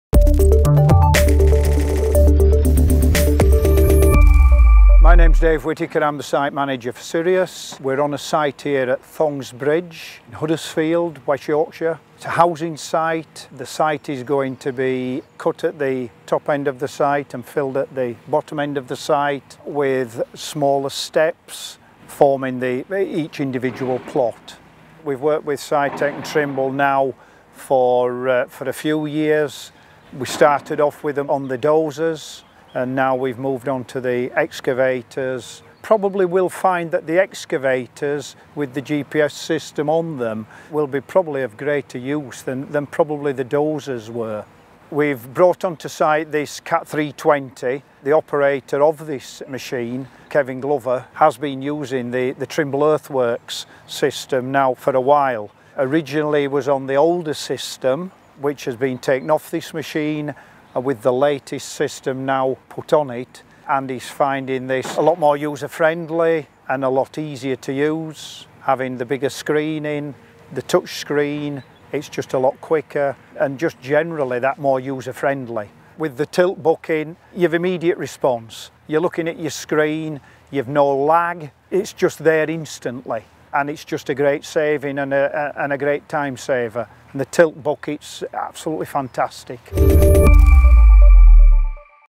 SITECH and Sirius Group first, with tilt bucket used on Trimble Earthworks automatics for Excavators
The Sirius Group are the first to use the Trimble Civil Engineering and Construction Earthworks automatics solution, complete with a tilt bucket.

See its Finning Equipment Solutions supplied Cat 320E in action here, creating benches on a house building site near Huddersfield and hear from site manager, Dave about his thoughts on the new system.

For more information on Trimble Earthworks for Excavators and other machine control or surveying solutions in the UK & Ireland